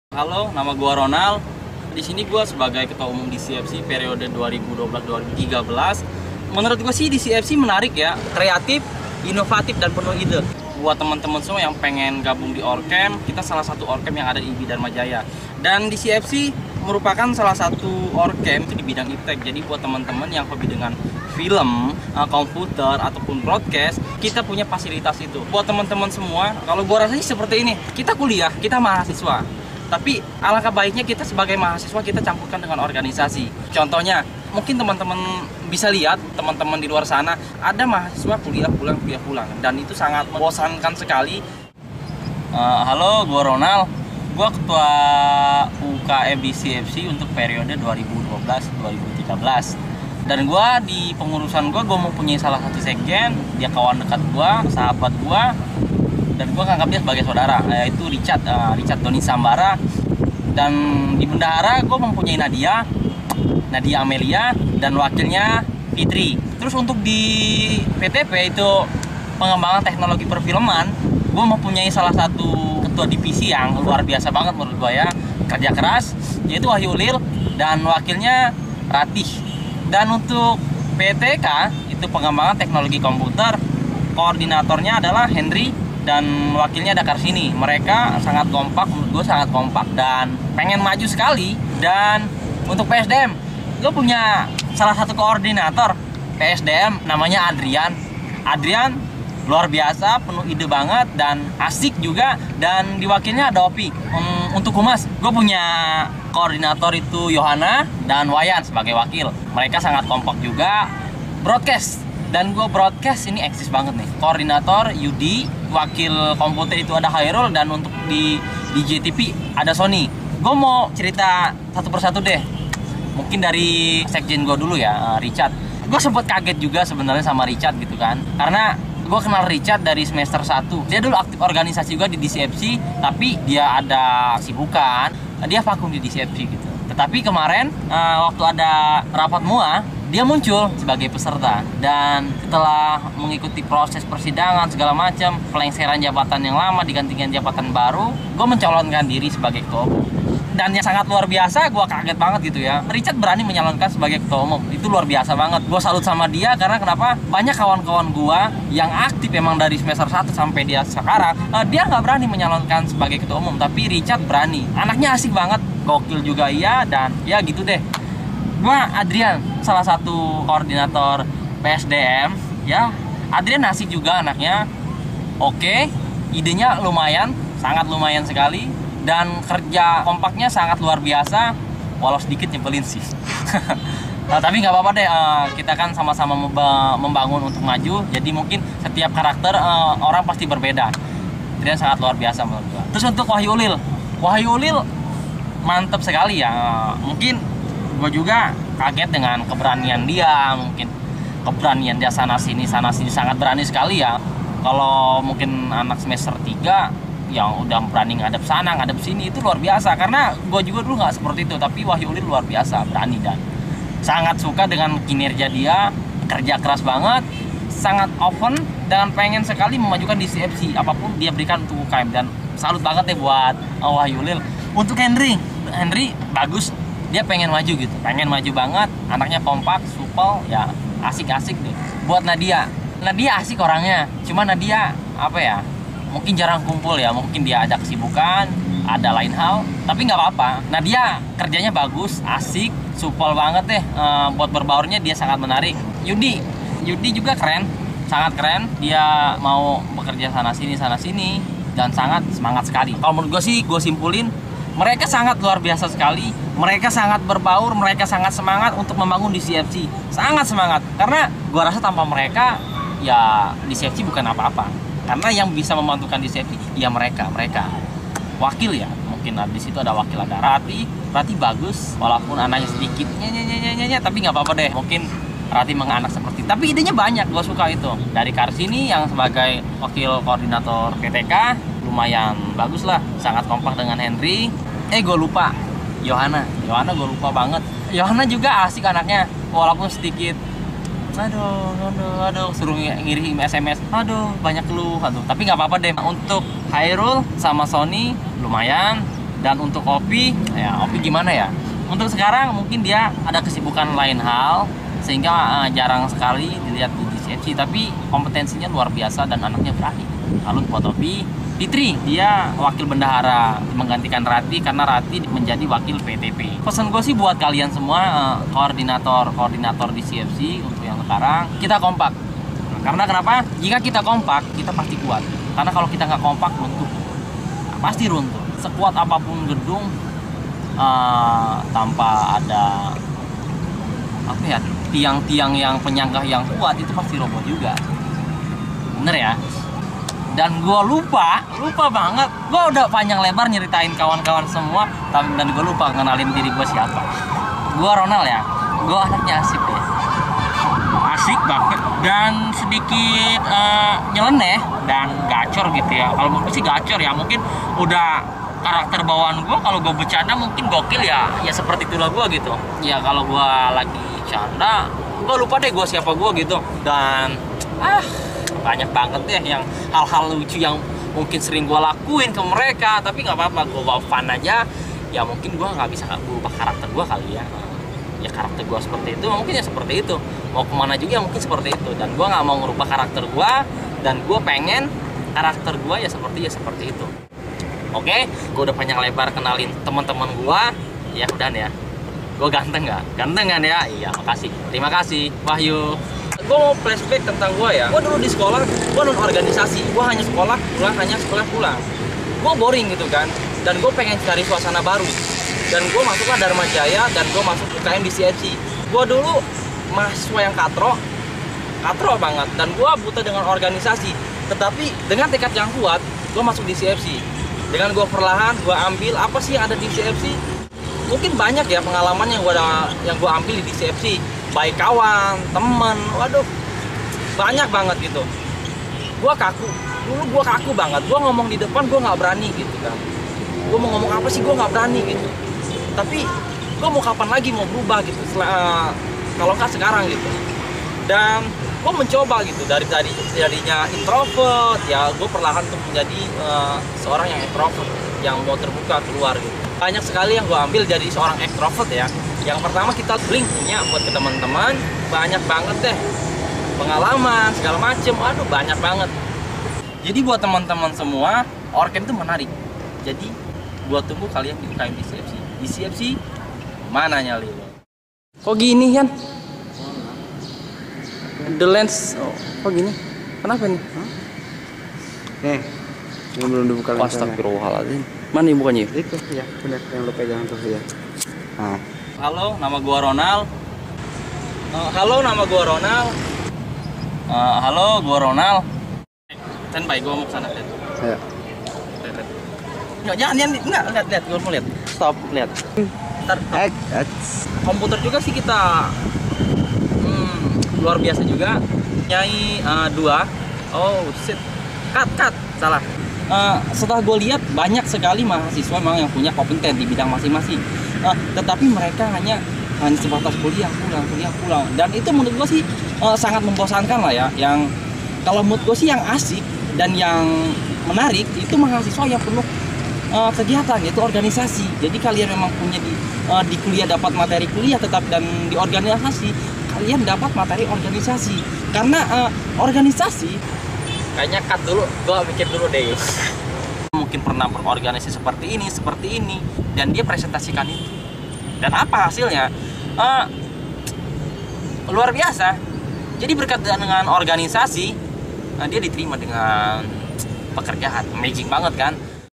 Pengenalan (detail) DCFC 2012-2013 by Ronal Sanjaya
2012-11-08 Pengenalan UKM DCFC tahun angkatan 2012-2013 by the Ketua Umum Ronal Sanjaya.
Ini sebenernya yang dikenalin secara mendetail adalah kru nya atau apa nya, atau malah beliau ini curhat kepada kita-kita sekalian?
Tolong ditonton dalam posisi sedang tidak emosi.
//Perhatian: Terdapat Behind The Scenes yang lumayan ngeselin.

Credit
Cast: Rona Sanjaya
Cameramen: Wahyu Ulil M.
Editor: Adrian Ihsan P.